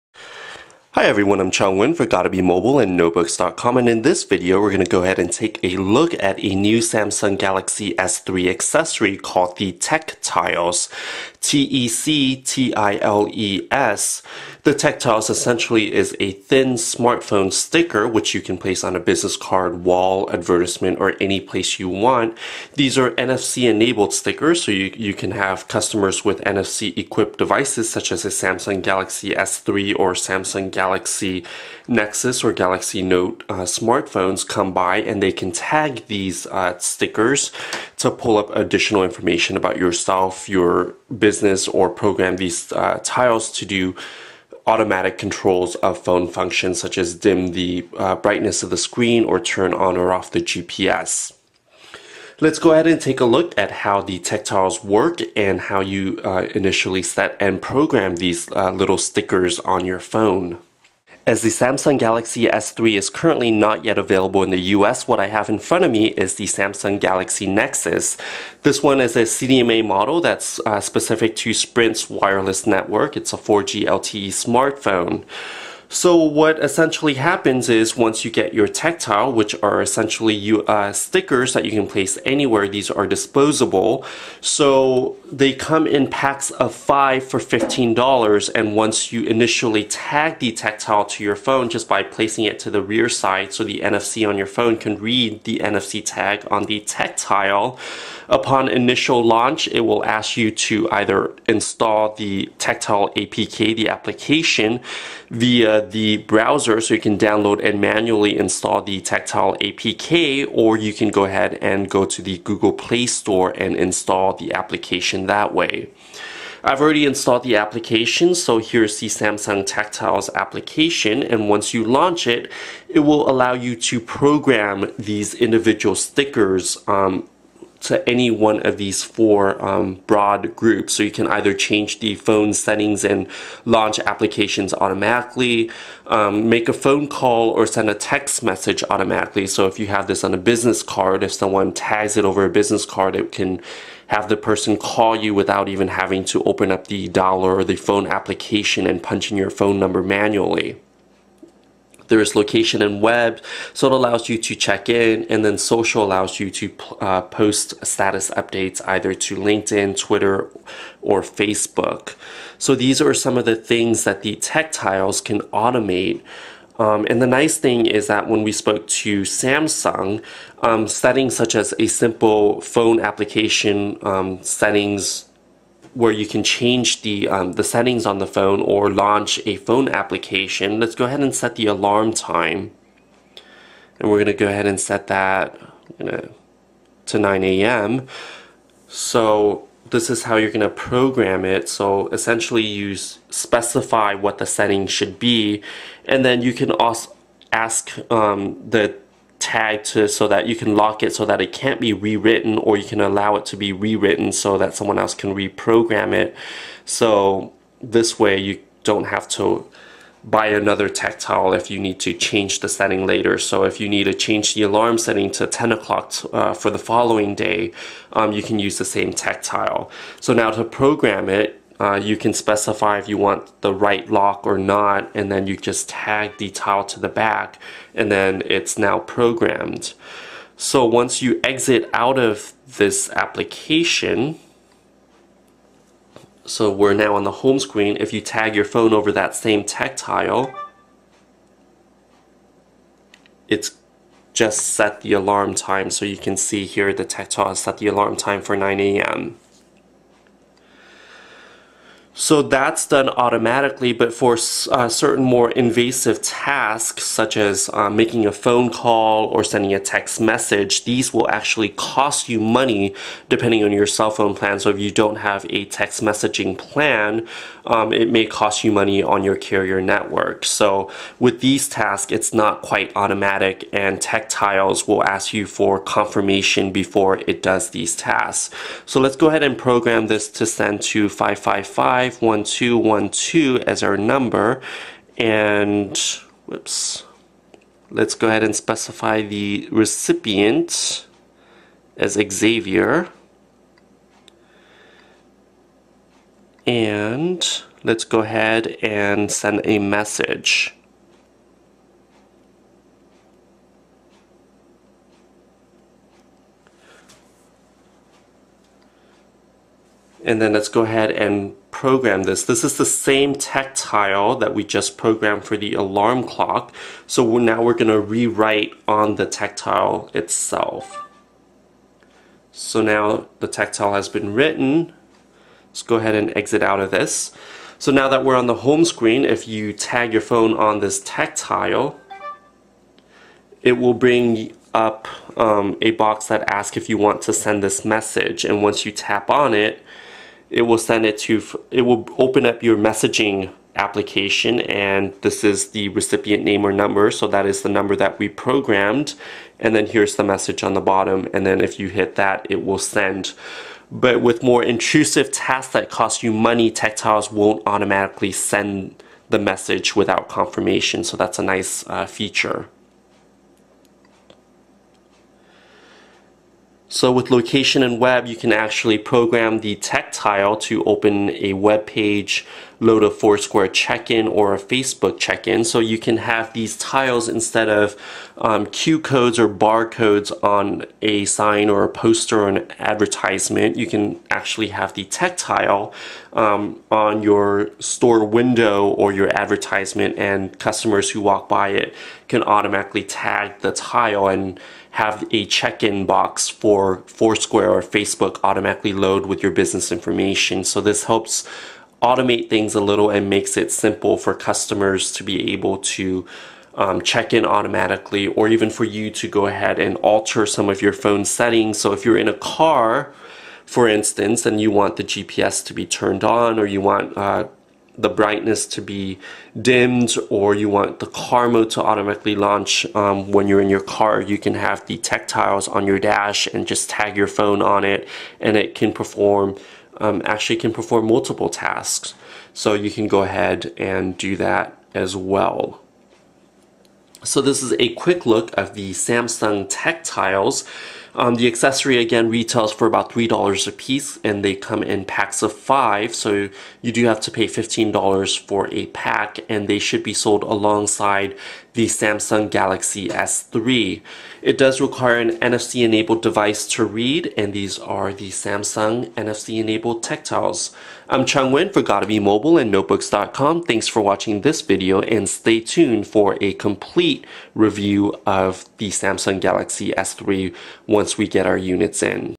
Обзор NFC-стикеров Samsung TecTiles (EAD-X11SWEGSTD)
Компания Samsung выпустила очень интересный аксессуар -- программируемые NFC метки Samsung TecTile, которыми можно управлять с помощью одноименного приложения.

Связка меток TecTile и программы позволяет управлять смартфоном, поддерживающим технологию NFC -- автоматически изменять настройки, запускать программы, отмечать свое местоположение, обновлять свой социальный статус и даже делать исходящие вызовы и отправлять текстовые сообщения.